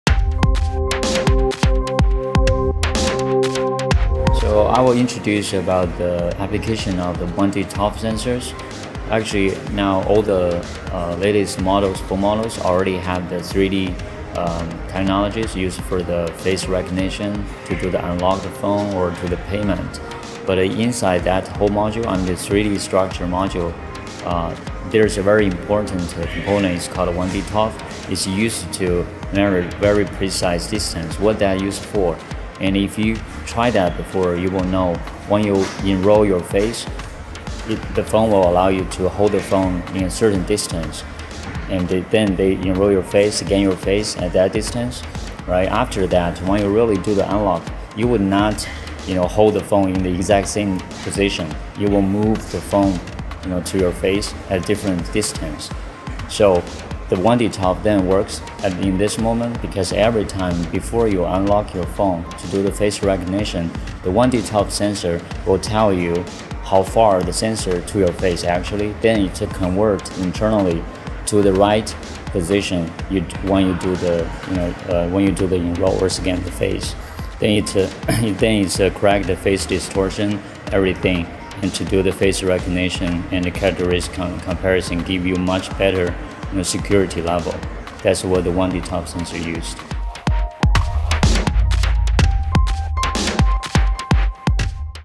1D Time of Flight Demo
Discover the world’s smallest 1D time-of-flight sensor for accurate proximity sensing and distance measurement in smartphones.

In this short video, Spencer Bai, ams Field Application Engineer, explains why the 1D-ToF sensor is the ideal companion to a smartphone’s face recognition system - for unlocking phones or mobile payment.